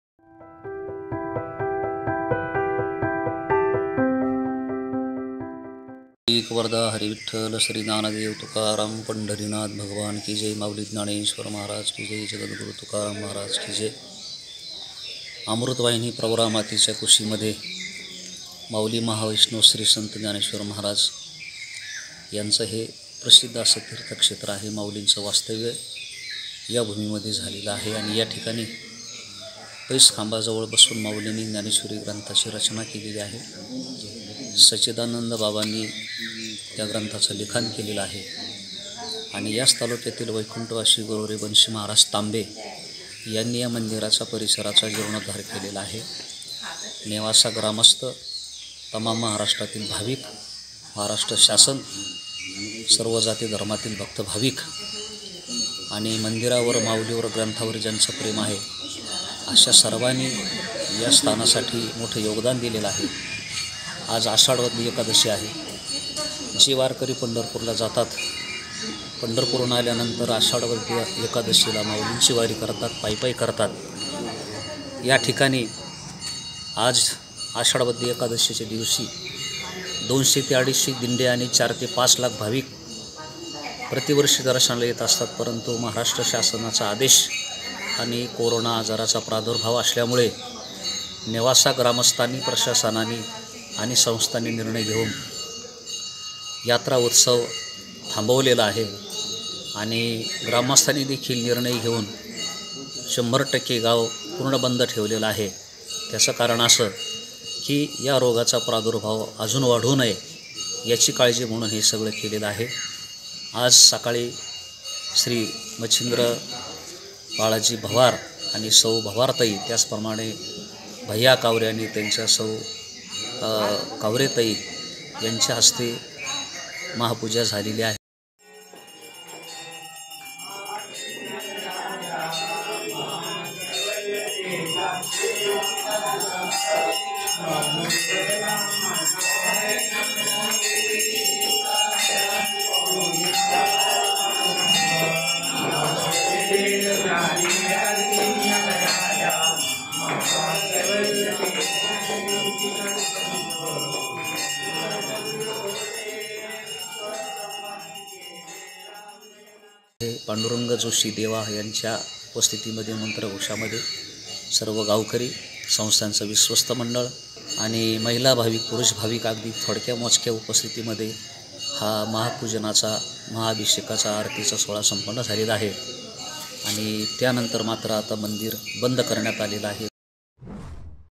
पैस खांबाच्या दर्शनाला मुकले लाखो वारकरी | newasa | ahmednagar
कामिका एकादशी निमित्त नेवासा येथे श्री संत ज्ञानेश्वर माऊलींच्या पैस खांबाचे दर्शनाला दरवर्षी लाखो भाविक येत असतात. यावर्षी करोनामूळे भाविक दर्शनाला मूकले.